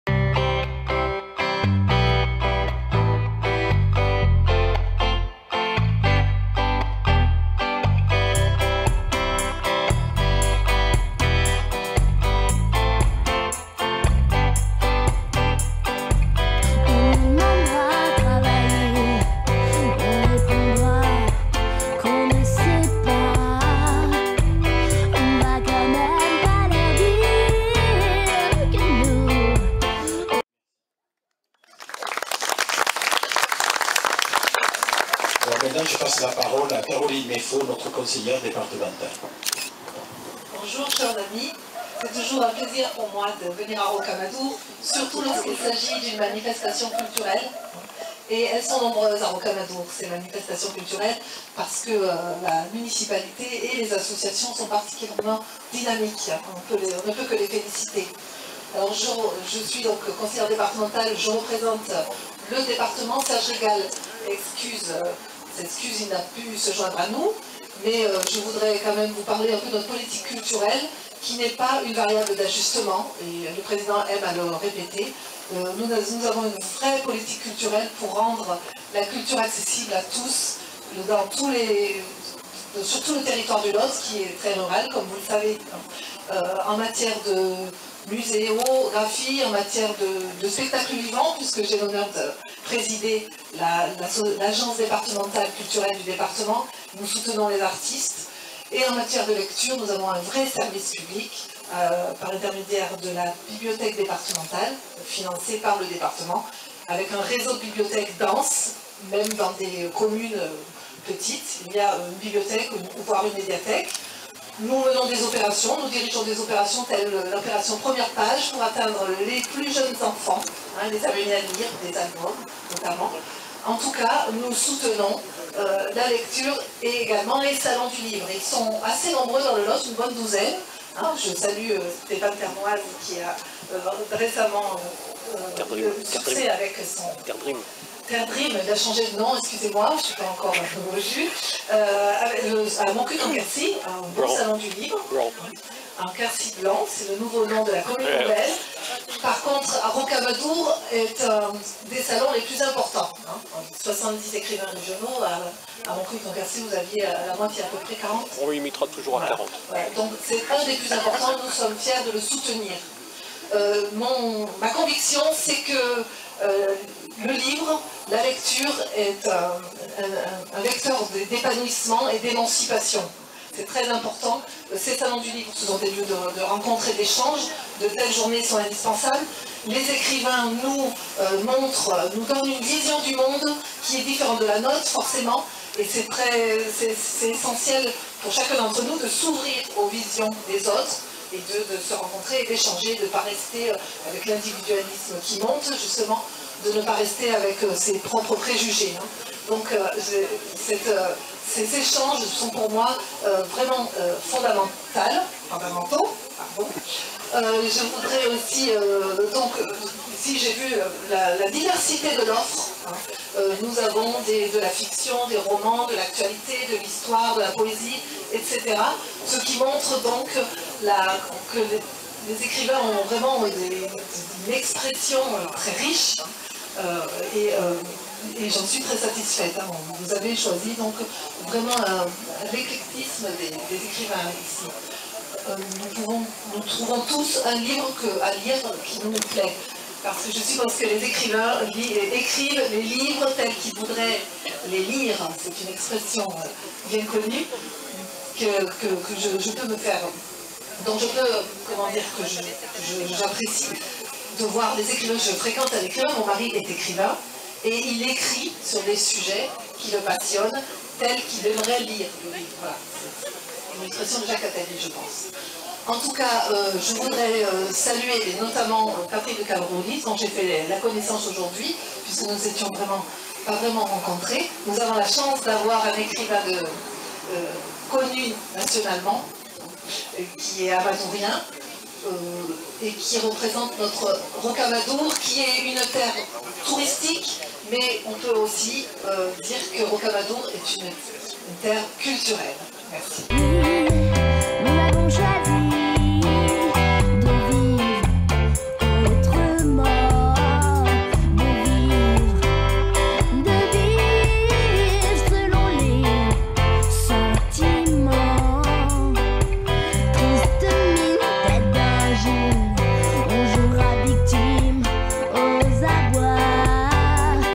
Caroline Mey-Fau Conseillère départementale du Lot 46 élue du canton Gramat Rocamadour Préside ADDA
Enregistré le 3 septembre 2017 à L'Hospitalet (section de Rocamadour ; Lot)
Au 9eme Salon du Livre.

Avec Mme Caroline Mey, élue départementale du Lot, Présidente de l'ADDA, nous avons échangé... mais l'interview publique, ce sera (sûrement) pour plus tard... Présidente ADDA

Aujourd'hui, il s'agit de ses propos lors de l'inauguration du salon du livre de Rocamadour le 3 septembre 2017...

Naturellement, je ne suis pas intervenu quand elle s'est adressée à moi... mais j'apporterai donc une précision (oui, il vous faut regarder pour savoir !!!)

Apparaissent : Huguette Tiegna, députée du Lot, Pascal Jallet, maire de Rocamadour, Stéphane Terdream